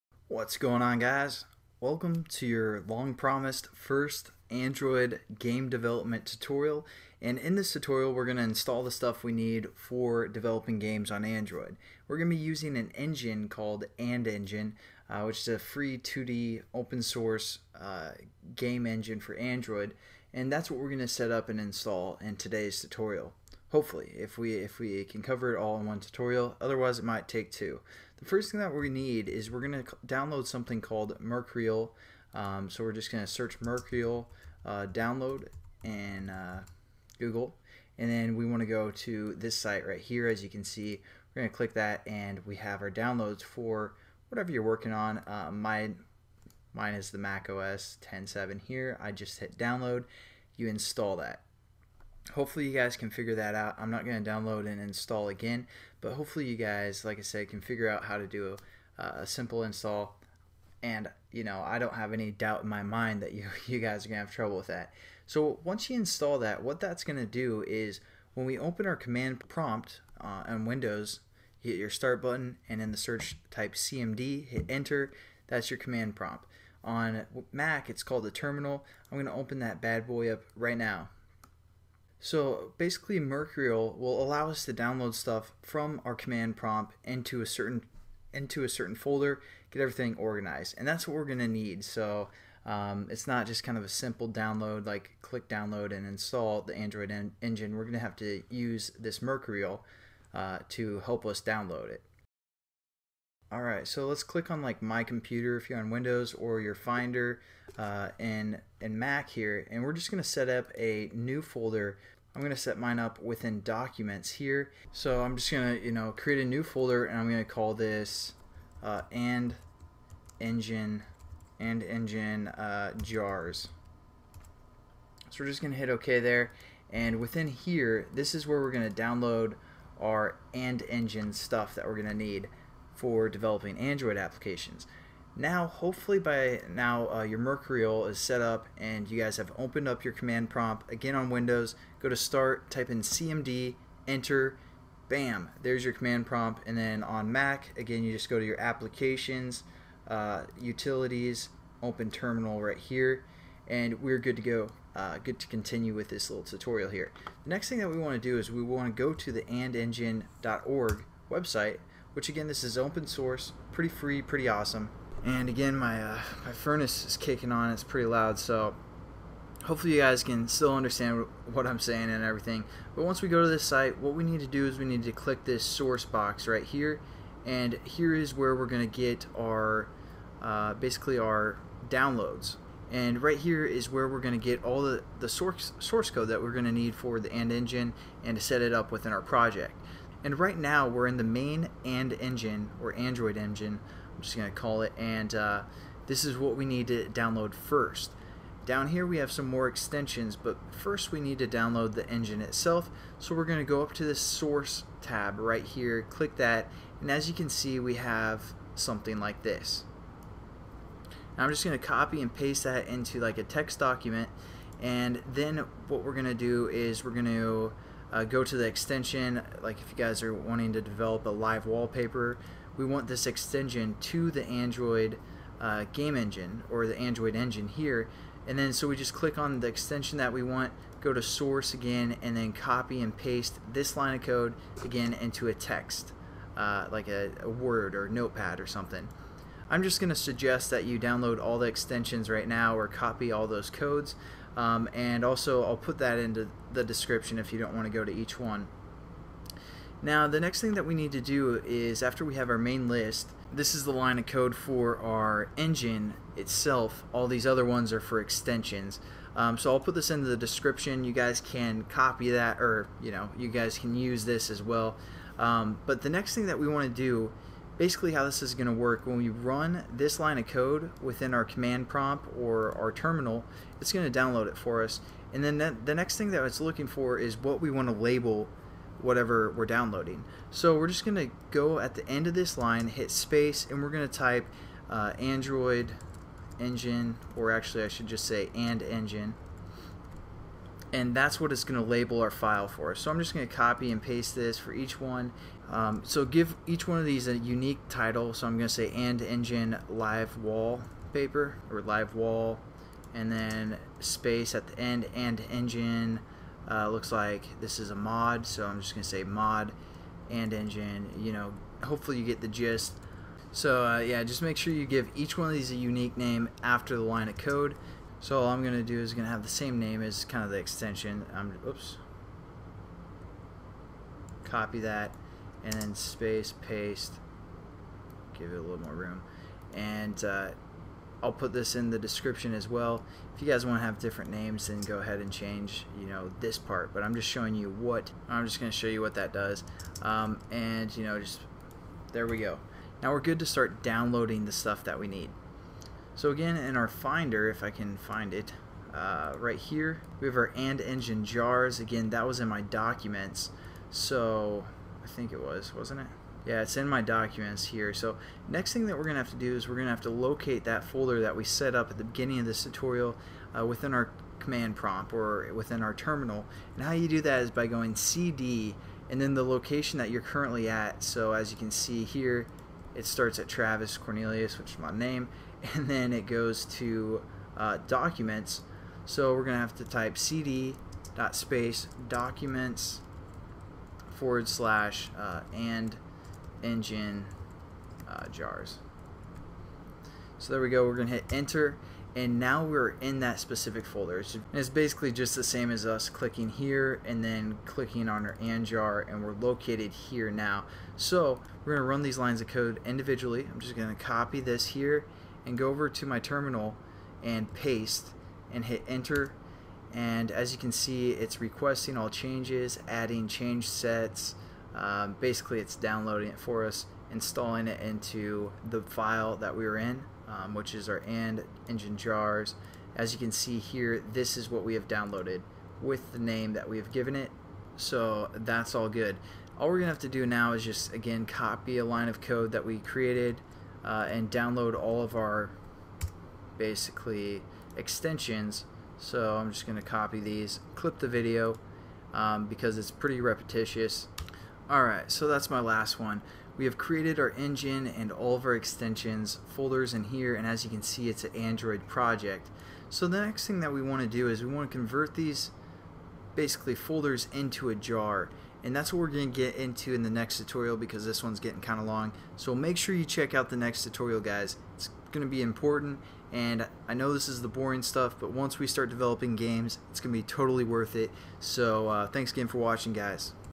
Android AndEngine Tutorial 1.1- Downloading AndEngine and Extensions
In this AndEngine tutorial we will learn about the android game engine.  AndEngine is a free open source 2d engine that we will use to create simple games for android.  AndEngine uses opengl es, so we can process graphics more efficiently.